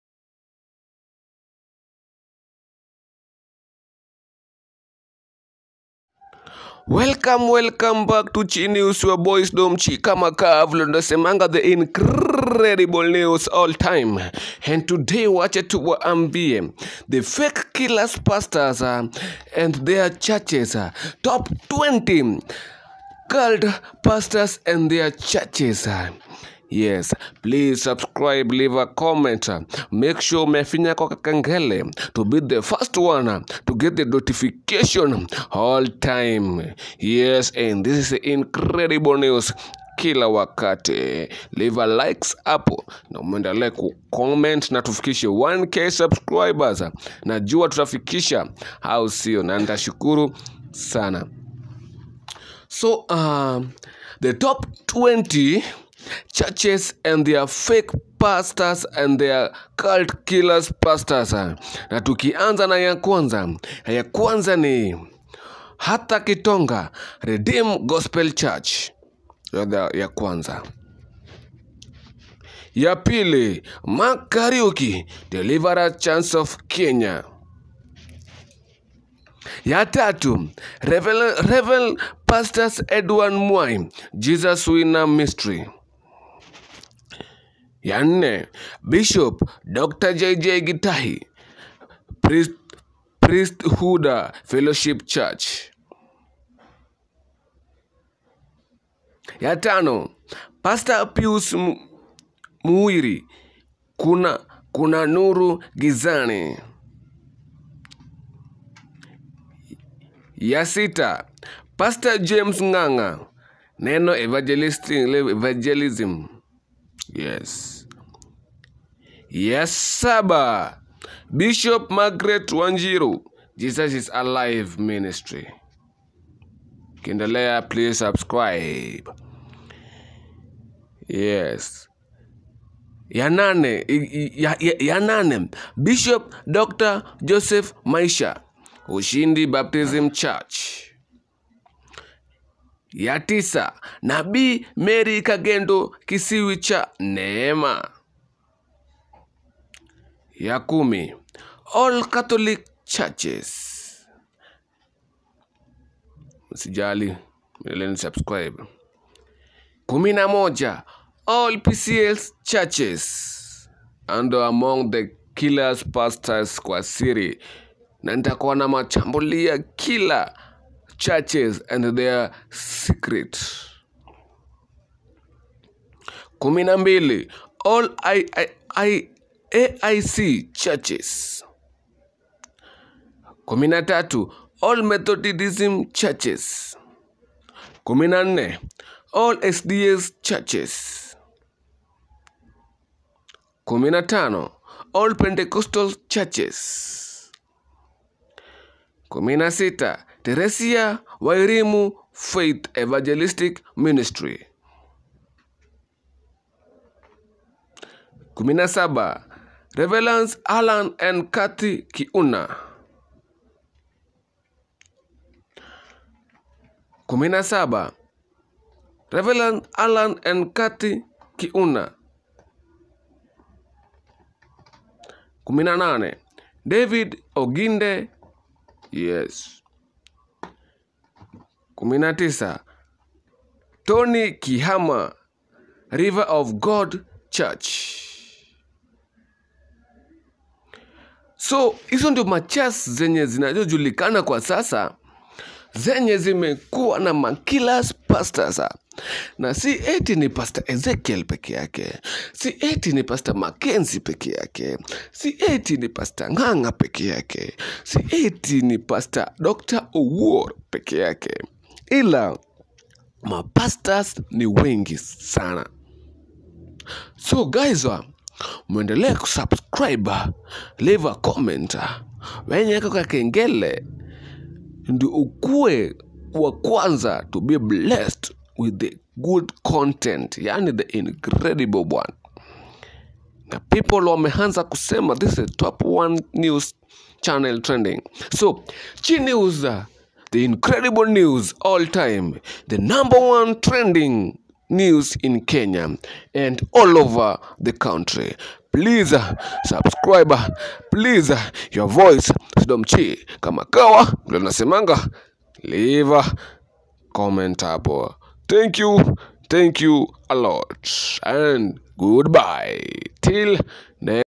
TOP 20 CULT FAKE KENYAN PASTOR'S AND THEIR CHURCHES
see the all stories of Pastor's that con people using the word of GOD